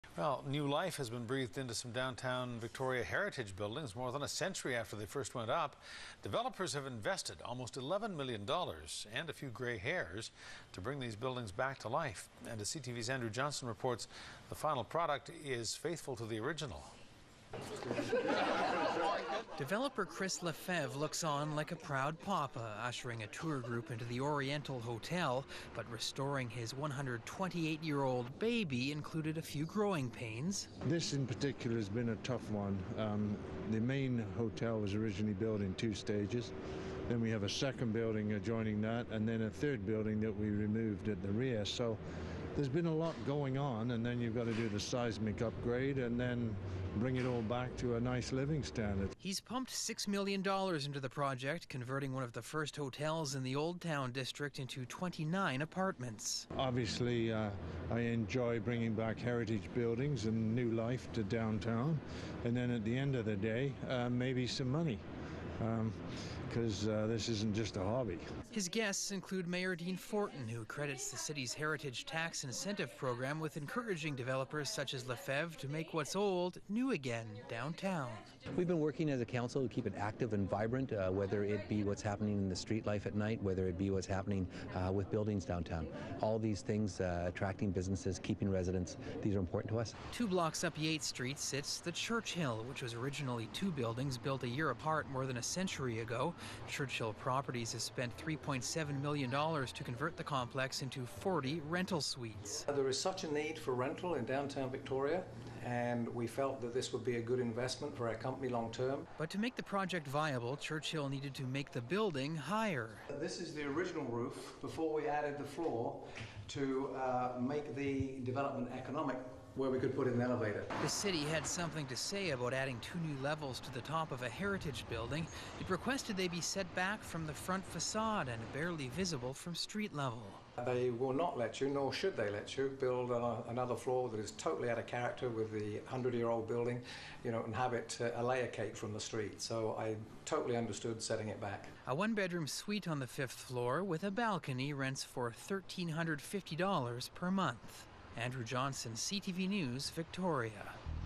New Life For Century Old Buildings
VICTORIA -- Developer Chris Le Fevre looks on like a proud papa Tuesday, ushering a tour group into The Oriental Hotel. But restoring his 128-year old 'baby' included a few growing pains. "This in particular has been a tough one. The main hotel was built in two stages, then a 2nd building was attached, and we removed a 3rd building out back. There's been a lot going on then a seismic upgrade and finally bringing it back to a nice living standard."

He's pumped $6 million into the project, converting one of the first hotels in the Old Town district into 29 apartments. "I enjoy bringing back heritage buildings and new life to downtown, and then at the end of the day maybe make some money because it isn't just a hobby" adds Le Fevre.

His guests include Mayor Dean Fortin who credits the city's heritage tax incentive program with encouraging developers such as Le Fefevre to make what's old new again downtown. "We've been working as a council to keep it active and vibrant, whether it be what's happening with street-life at night or whether it be what's happening with buildings downtown. All those things attracting businesses and keeping residents is important to us."

Two blocks up Yates Street sits The Churchill which was originally two buildings built a year apart more than a century ago. Churchill Properties has spent $3.7 million to convert the complex into 40 rental suites. "There's such a need for rentals in downtown Victoria we thought it would be a good long-term investment" says CEO Philip Langridge of Churchill Properties.

But to make the project viable, Churchill needed to make the building higher. The City had something to say about adding two new levels to the top of a heritage building. It requested they be set back from the front façade, and barely visible from street-level. "They will not let you nor should they let you build another floor that's out of character with a 100 year old building. So I totally understood setting it back" Langridge adds.

A one-bedroom suite on the 5th floor with a large balcony rents for $1350 per month.